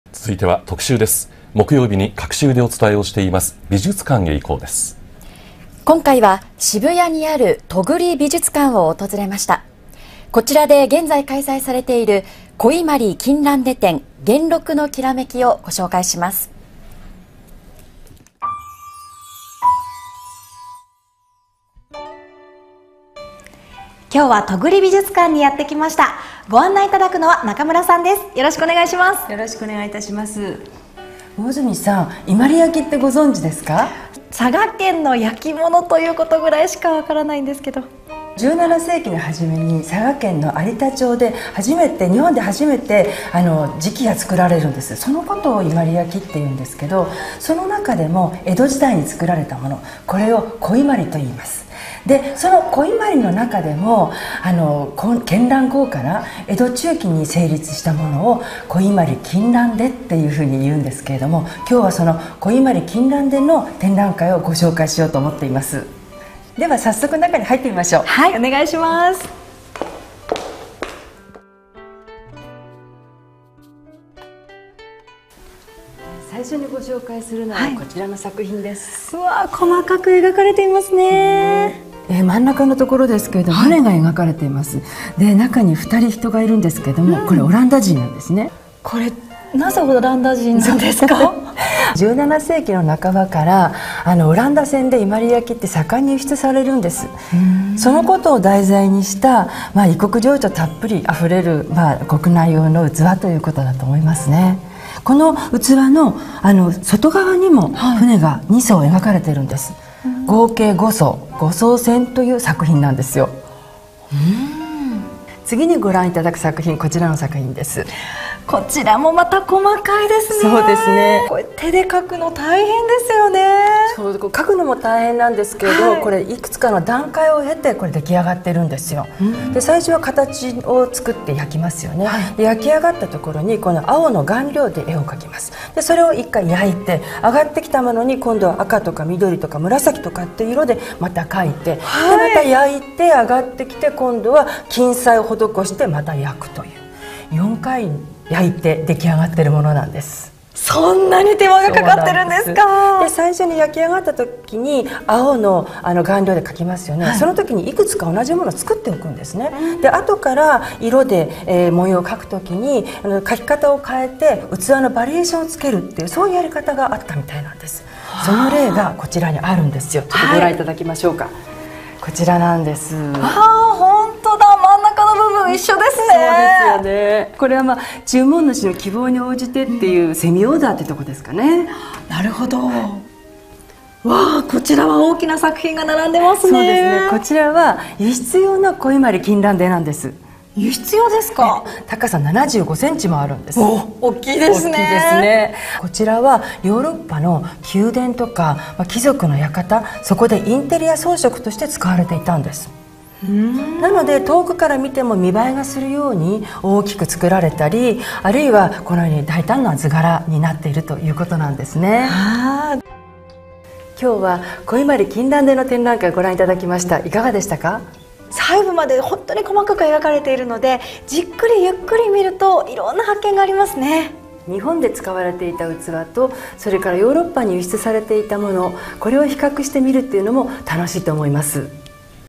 ［美術館へ行こう］ 戸栗美術館『古伊万里金襴手展』
首都圏の美術館情報をお伝えする［美術館へ行こう］です。今回は渋谷にある戸栗美術館で開催中の『古伊万里金襴手展　−元禄のきらめき−』を紹介します。

※『古伊万里金襴手展　−元禄のきらめき−』
戸栗美術館　2013年6月23日（日）まで
■■↓全文を見る■■